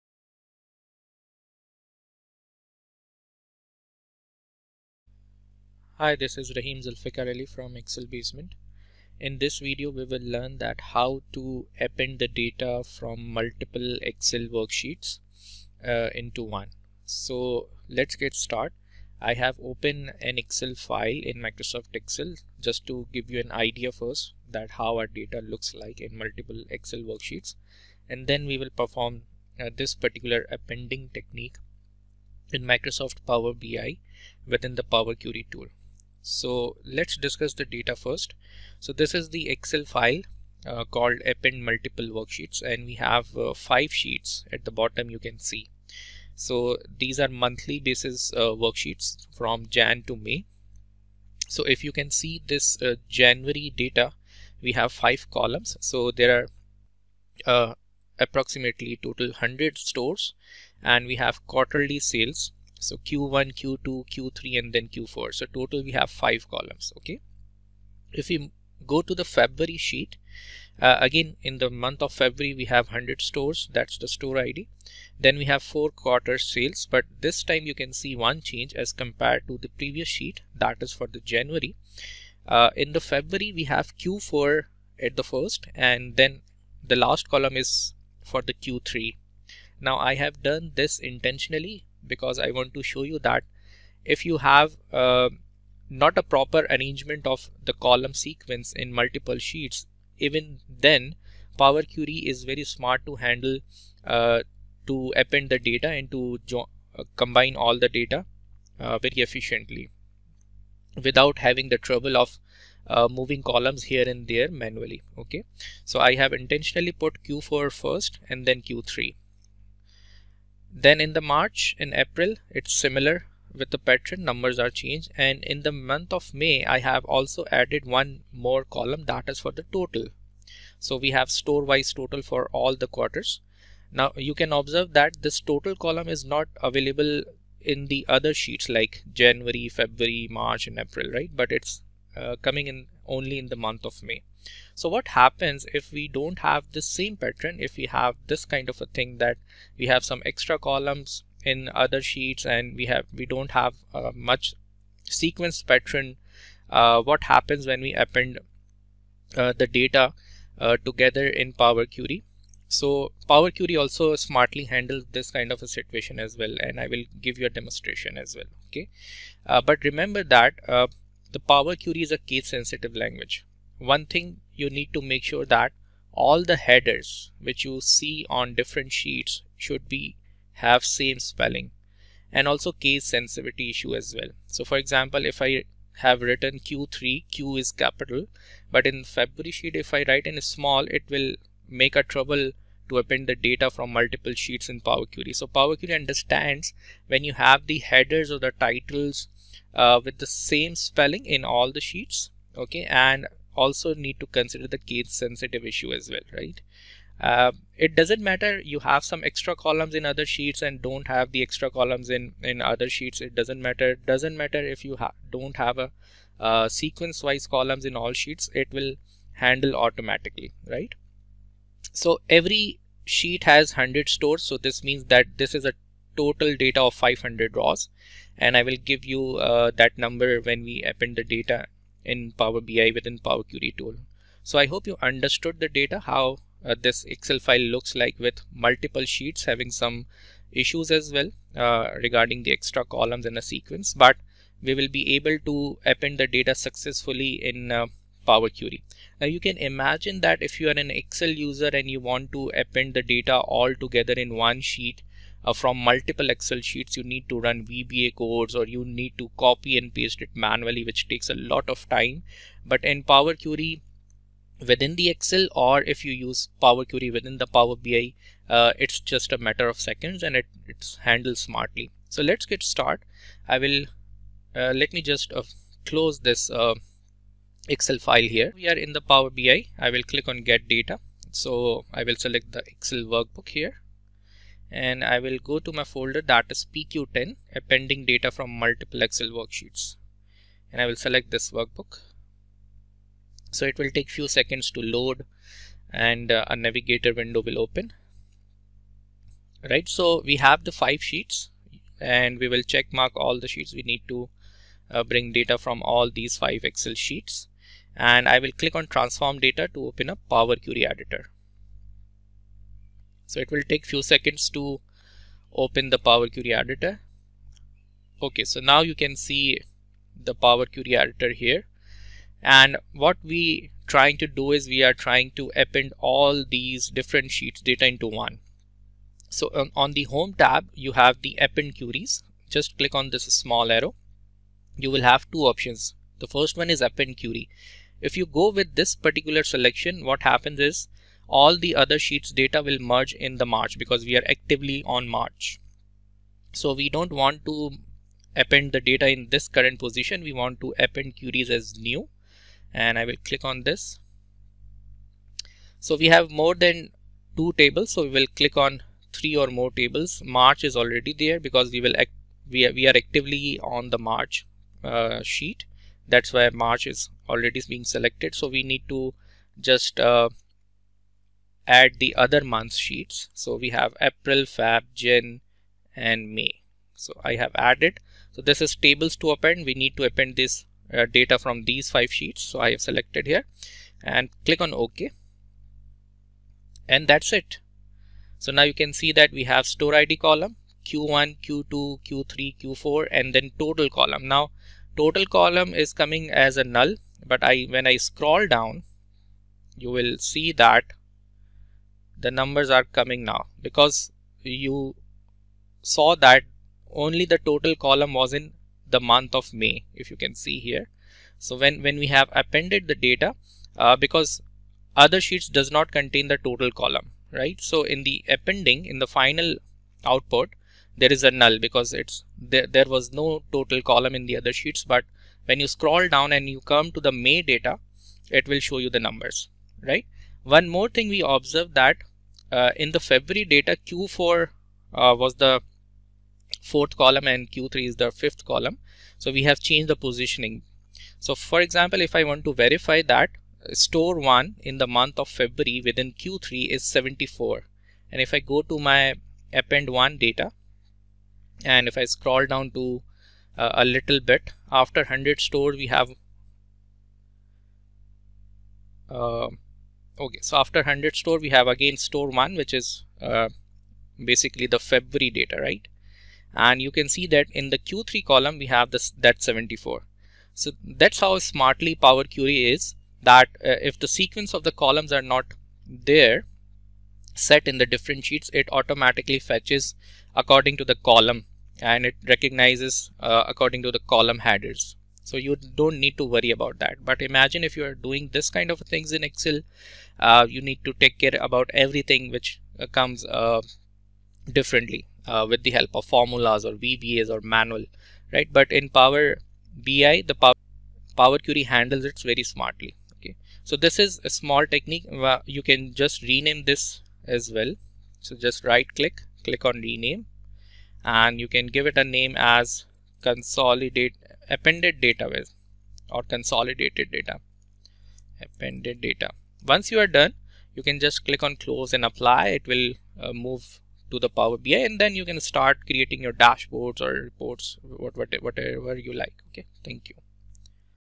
PQ 10 - Appending Data from Multiple Excel Worksheets | Microsoft POWER BI | ETL Tool Power Query
Eloquens is the world’s first marketplace to share top-tier best-practices for professionals. Don’t Reinvent The Wheel!